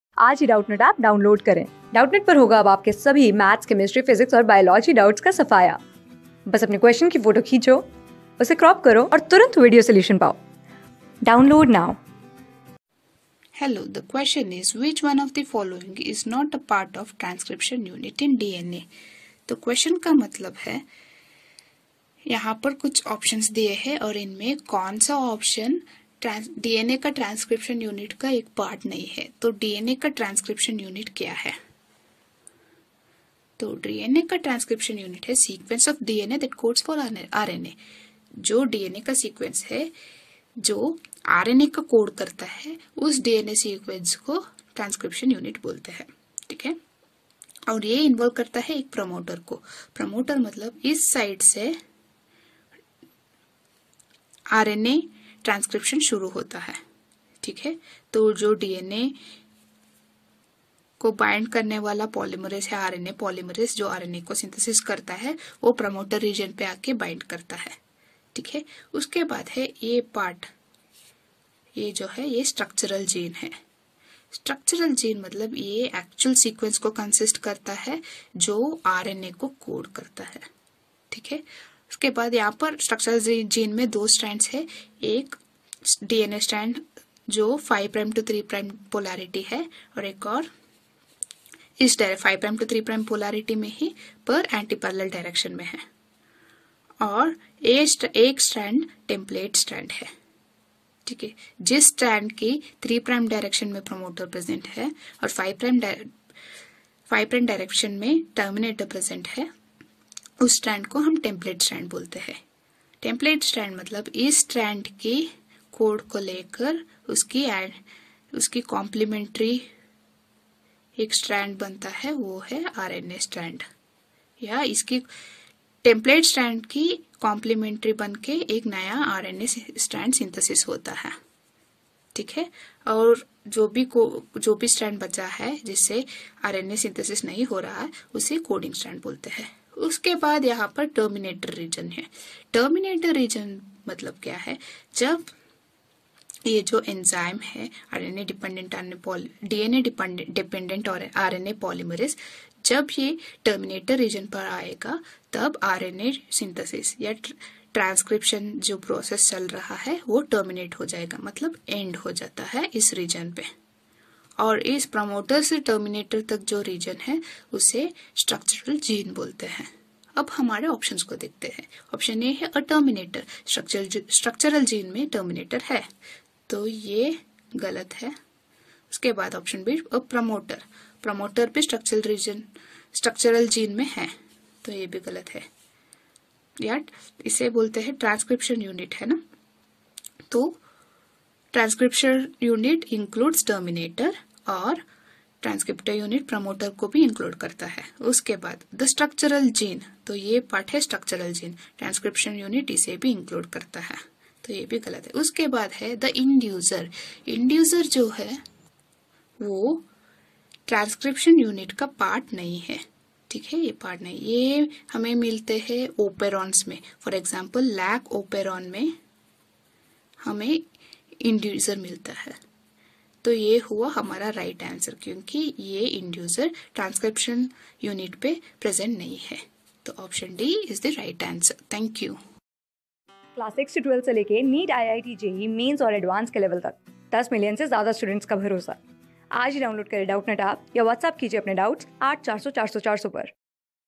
Which one of the following is not a part of a transcription unit in DNA? | CLASS 12 | MOLECULAR ...
Which one of the following is not a part of a transcription unit in DNA?
Class: 12
Subject: BIOLOGY
Chapter: MOLECULAR BASIS OF INHERITANCE
Board:IIT JEE

👉 Have Any Query? Ask Us.